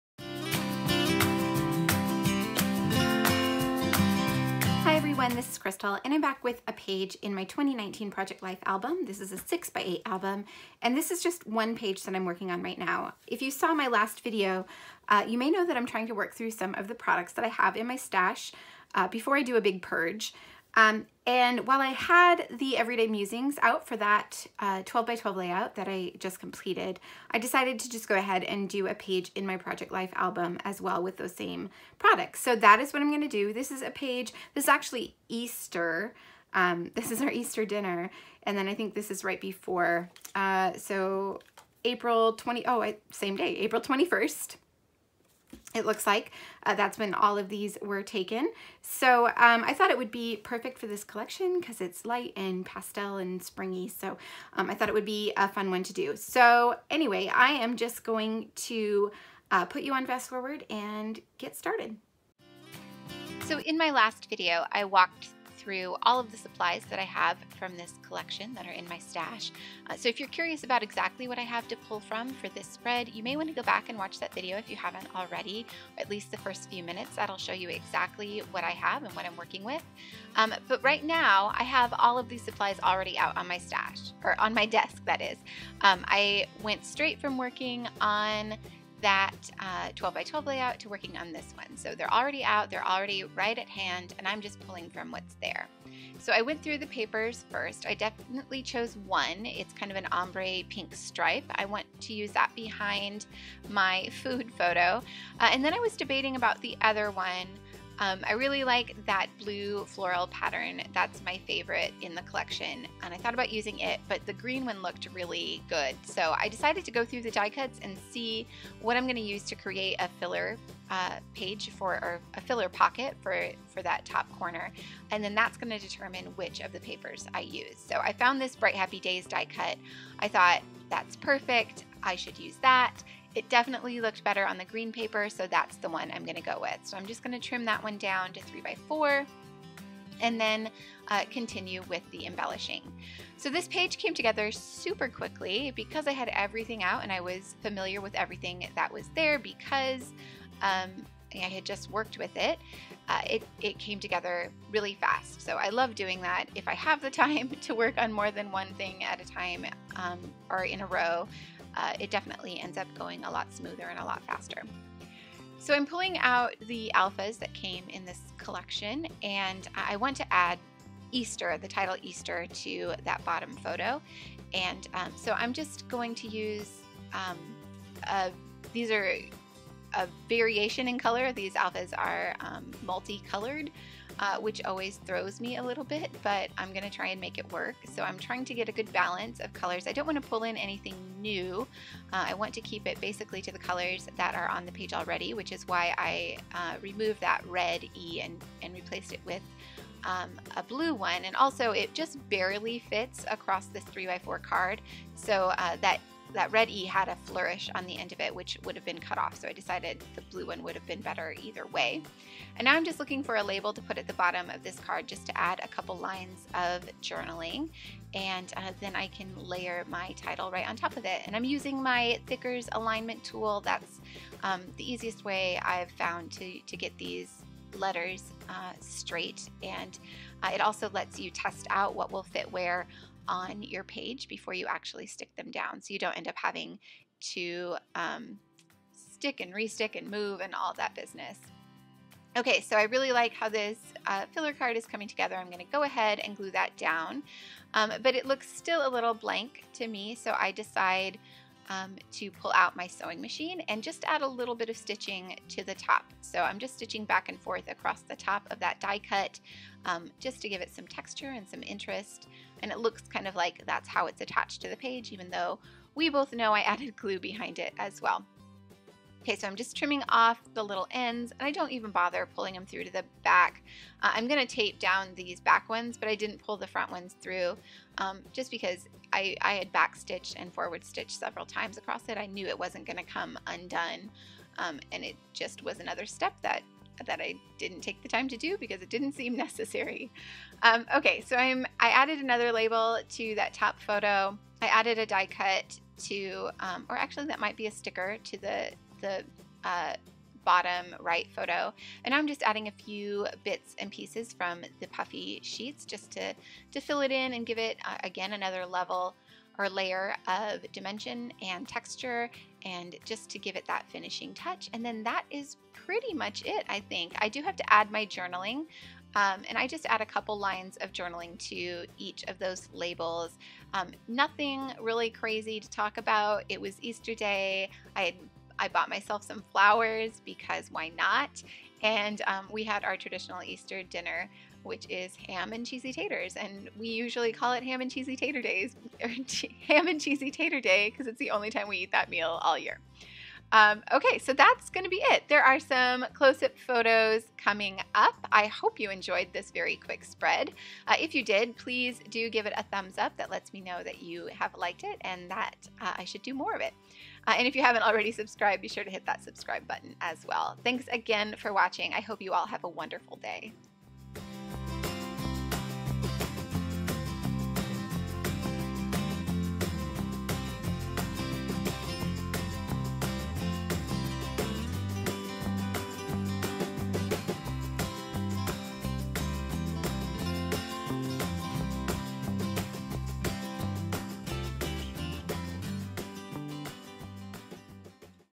2019 Project Life: April
Today I'm playing with the Everyday Musings collection from Pinkfresh Studio!

---Products Used---